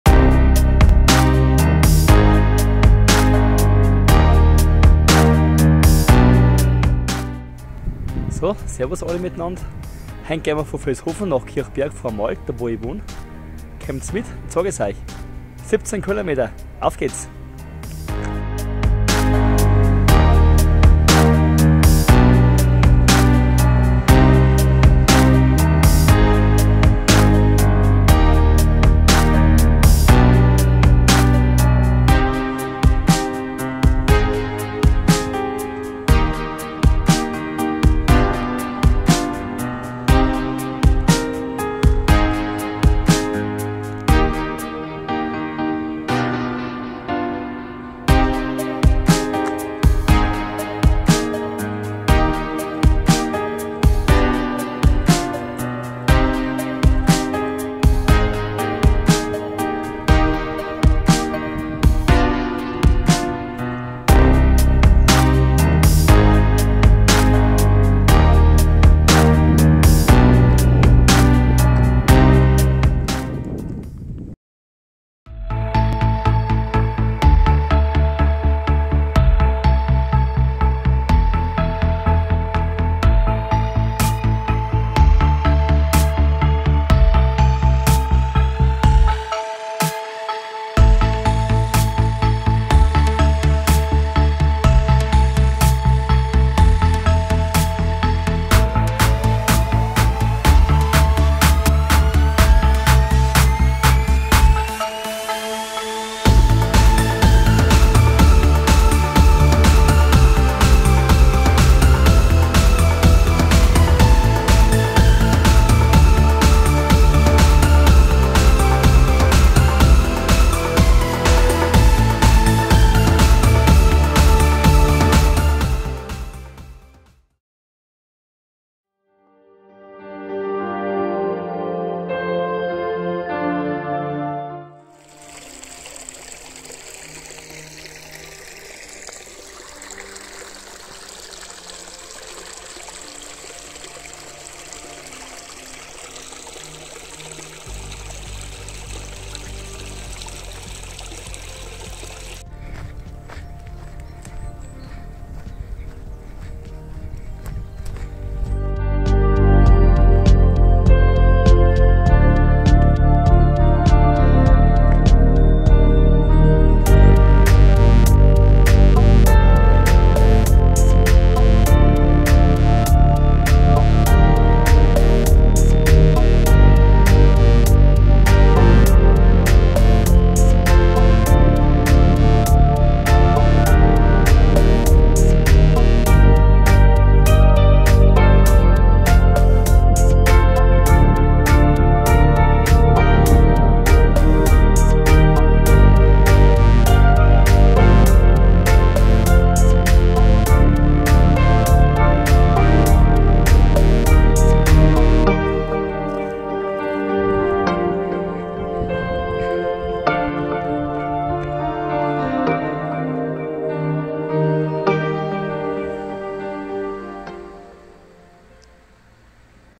Wanderung von Vilshofen nach Kirchberg vom Wald.
Über Windorf, Otterskirchen und dem Ebersberg